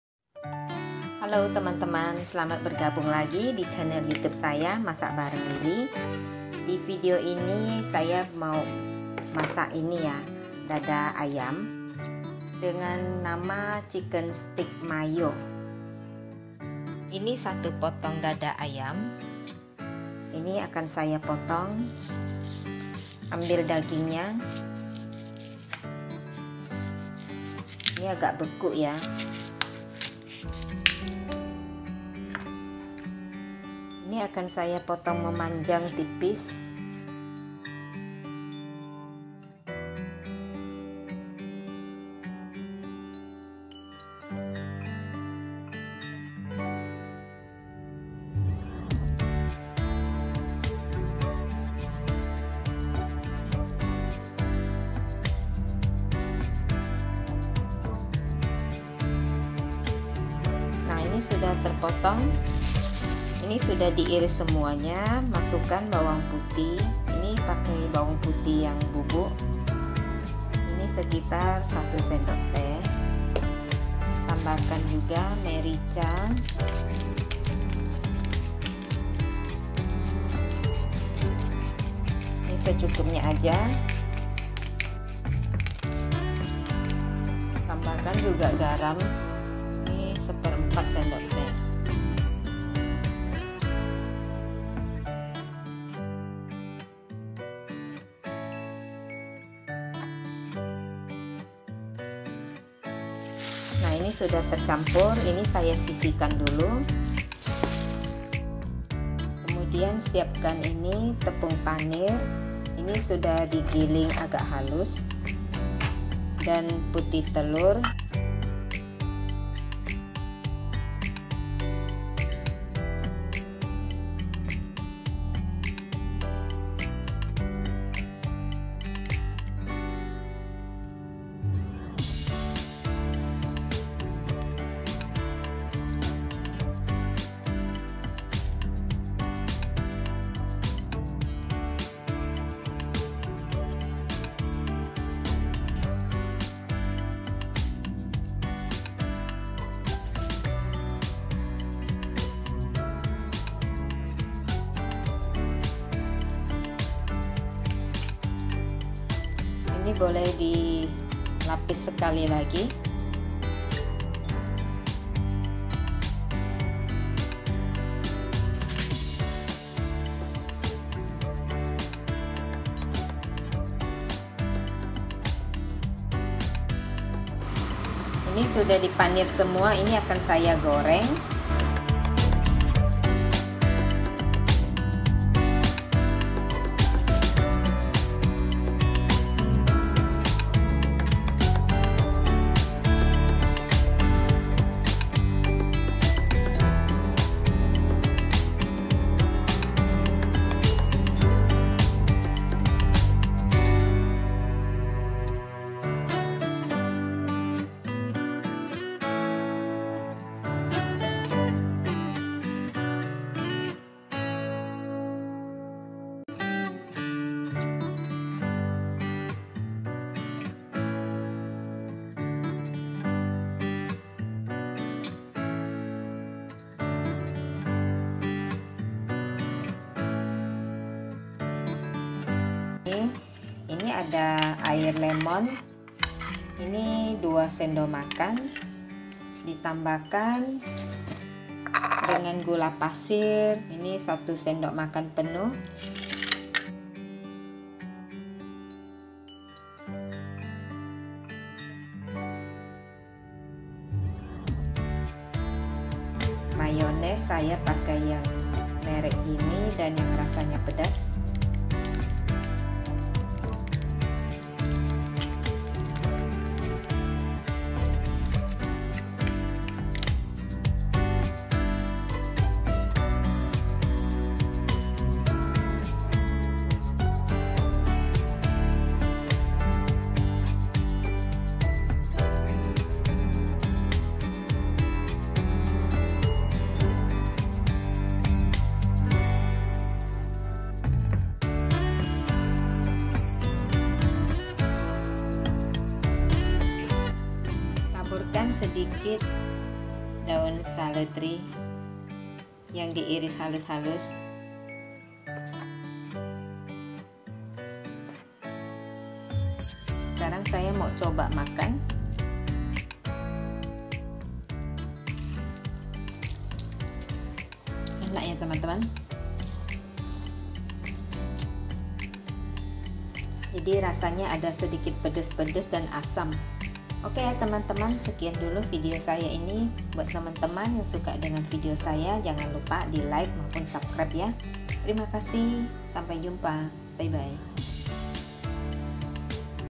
Chiken Stik Mayo
bahan2:
dada ayam
bawang putih
lada
garam
minyak goreng

paniran:
tepung roti
putih telu

mayonaise(bebas merk apa saja)
ait lemon
gula